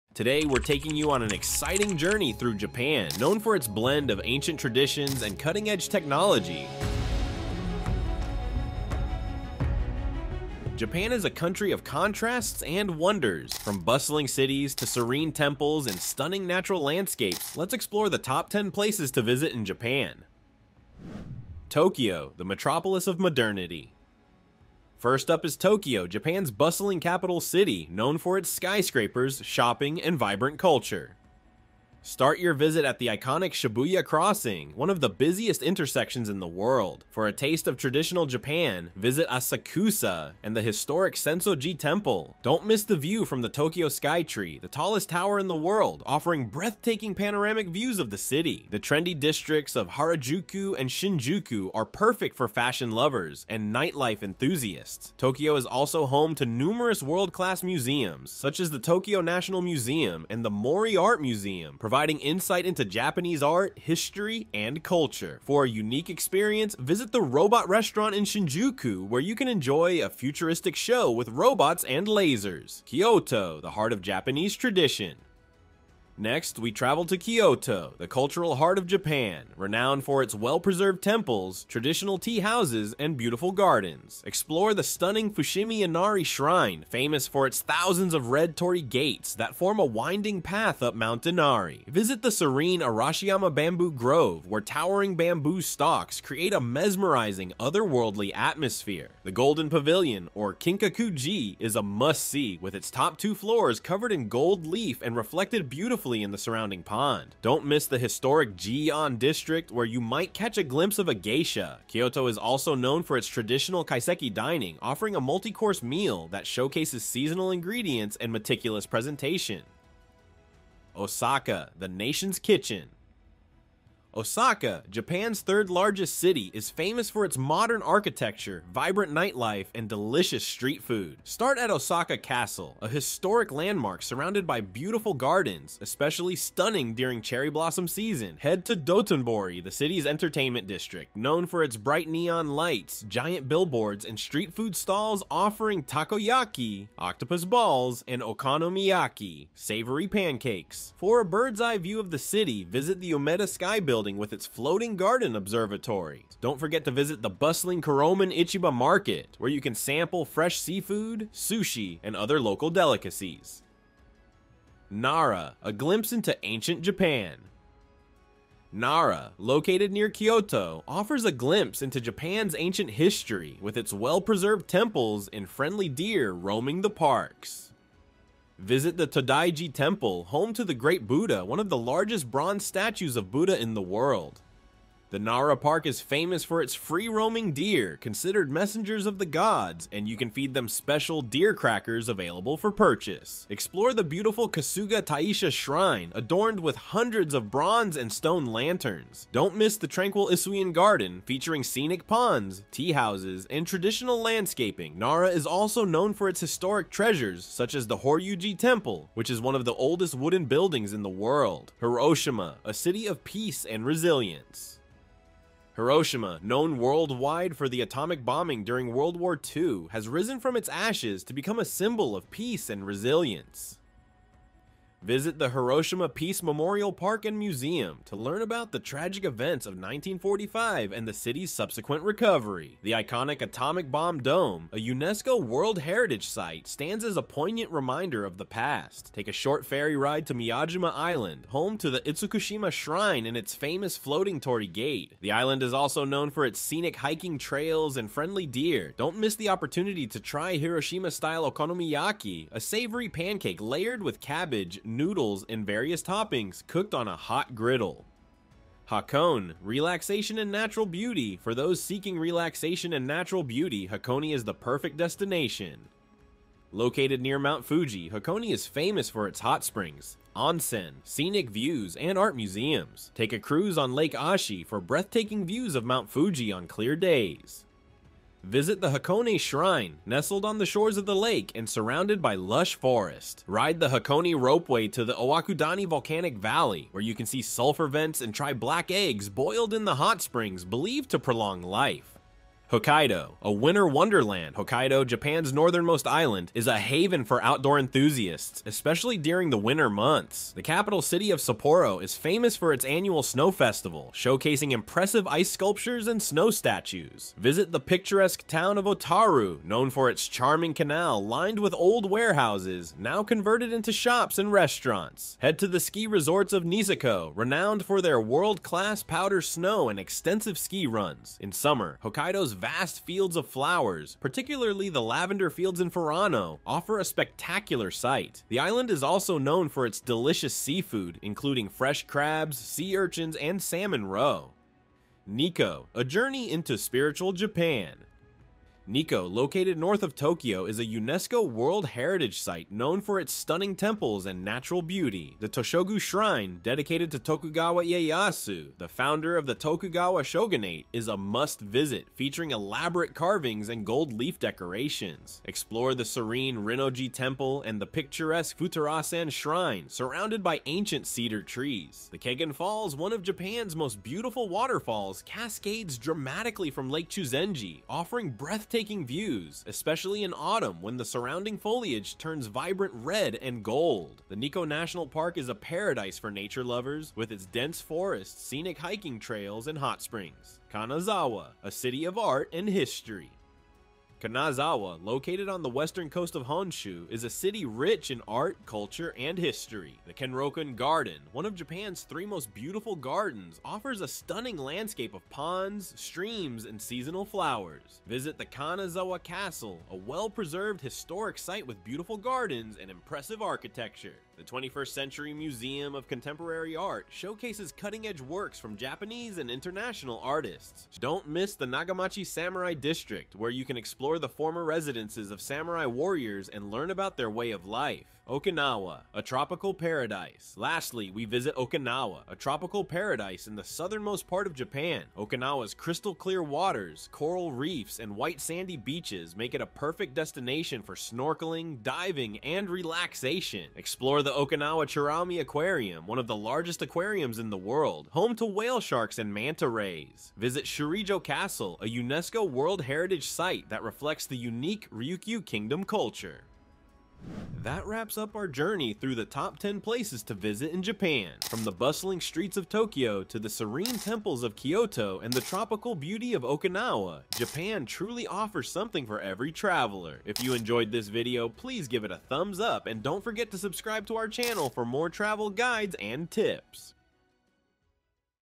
Japan Adventure: Must-See Places Unveiled!
Today, we're taking you on an exciting journey through Japan, a country renowned for its blend of ancient traditions and cutting-edge technology. From bustling cities to serene temples and stunning natural landscapes, Japan offers something for every traveler. Join us as we explore the top 10 places to visit in Japan!

📍 Featured Destinations:

Tokyo: The Metropolis of Modernity
Explore Japan’s bustling capital city, known for its skyscrapers, vibrant culture, and iconic landmarks. Don't miss Shibuya Crossing, Sensō-ji Temple, Tokyo Skytree, Harajuku, Shinjuku, and world-class museums.

Kyoto: The Heart of Japanese Tradition
Discover Kyoto's well-preserved temples, traditional tea houses, and beautiful gardens. Visit Fushimi Inari Shrine, Arashiyama Bamboo Grove, Kinkaku-ji (Golden Pavilion), and the historic Gion district.

Osaka: The Nation's Kitchen
Experience Osaka’s vibrant nightlife, delicious street food, and modern architecture. Explore Osaka Castle, Dotonbori, Umeda Sky Building, Universal Studios Japan, and Kuromon Ichiba Market.

Nara: A Glimpse into Ancient Japan
Visit Nara's ancient temples and friendly deer. Highlights include Todai-ji Temple, Nara Park, Kasuga Taisha Shrine, and Isuien Garden.

Hiroshima: A City of Peace and Resilience
Learn about Hiroshima's history and resilience. Visit Hiroshima Peace Memorial Park, Atomic Bomb Dome, Miyajima Island, and try Hiroshima-style okonomiyaki.

Hakone: Relaxation and Natural Beauty
Relax in Hakone's hot springs and enjoy scenic views of Mount Fuji. Take a cruise on Lake Ashi, visit Hakone Shrine, and ride the Hakone Ropeway to Owakudani volcanic valley.

Hokkaido: A Winter Wonderland
Explore Hokkaido’s outdoor activities and stunning landscapes. Visit Sapporo, Otaru, Niseko ski resorts, and Furano's lavender fields. Enjoy Hokkaido's delicious seafood.

Nikko: A Journey into Spiritual Japan
Discover Nikko's stunning temples and natural beauty. Visit Toshogu Shrine, Rinno-ji Temple, Futarasan Shrine, Kegon Falls, and Nikko National Park.

Kanazawa: A City of Art and History
Experience Kanazawa's rich culture and history. Visit Kenrokuen Garden, Kanazawa Castle, 21st Century Museum of Contemporary Art, Higashi Chaya District, and Nagamachi Samurai District.

Okinawa: A Tropical Paradise
Relax in Okinawa's tropical paradise. Explore Okinawa Churaumi Aquarium, Shurijo Castle, Okinawa Peace Memorial Park, and enjoy local cuisine and vibrant festivals.